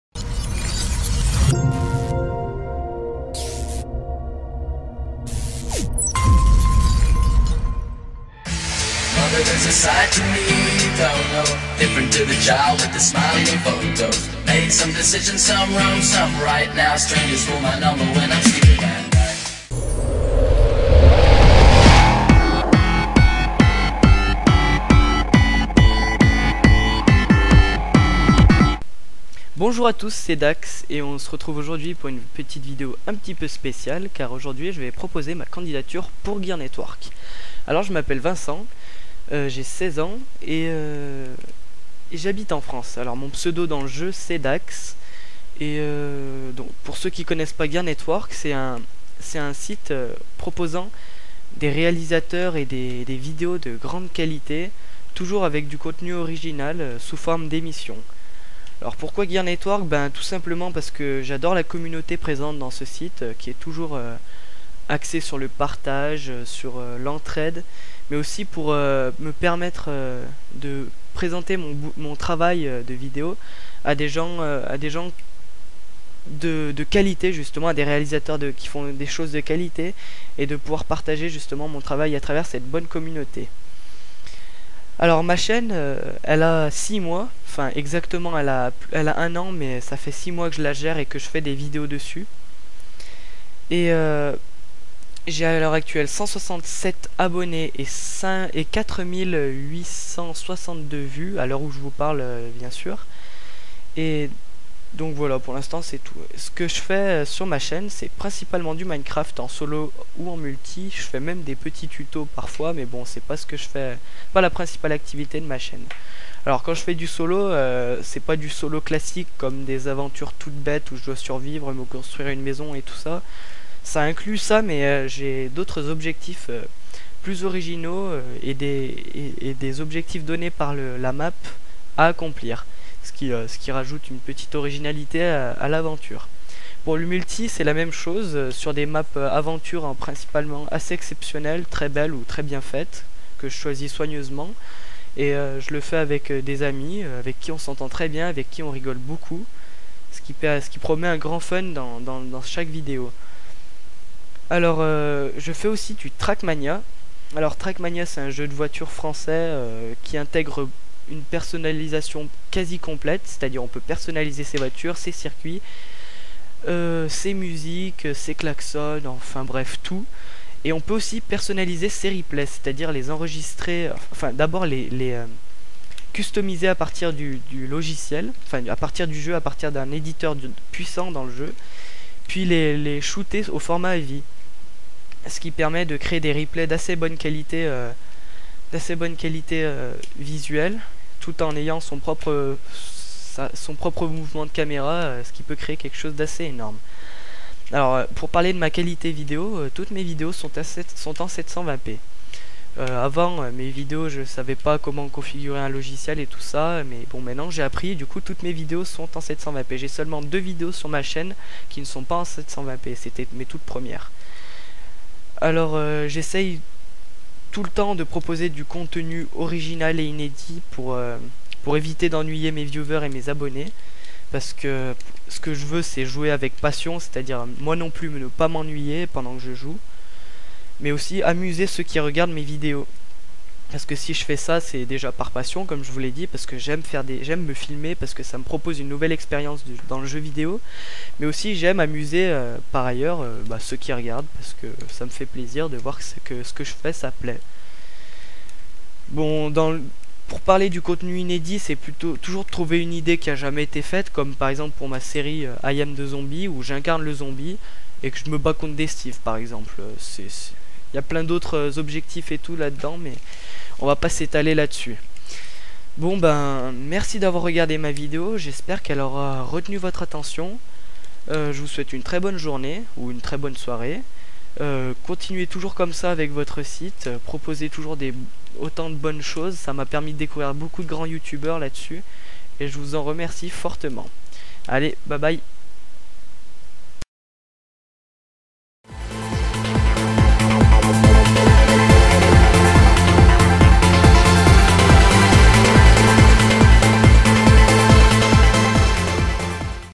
► Candidature Gear network de Daxx ! HD 720p
► Description ! ◄
∟Pour cette petite vidéo spéciale, je propose ma candidature pour gear network ! :)